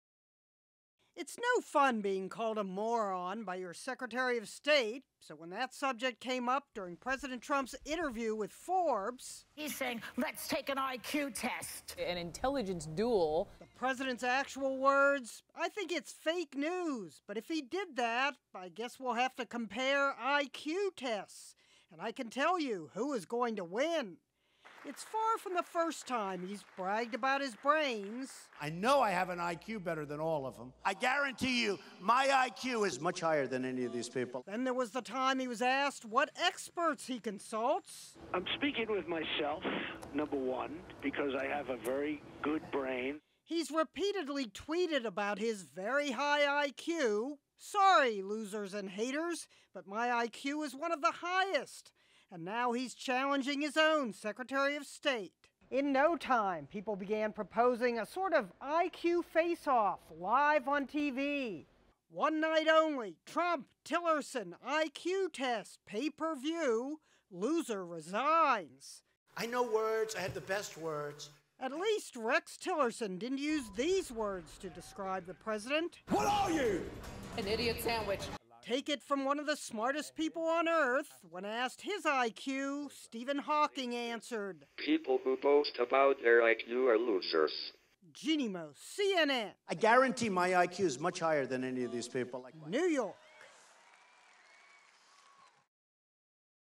Trump: I have a very good brain
Watch Donald Trump brag about his brain power. CNN's Jeanne Moos reports on the IQ showdown.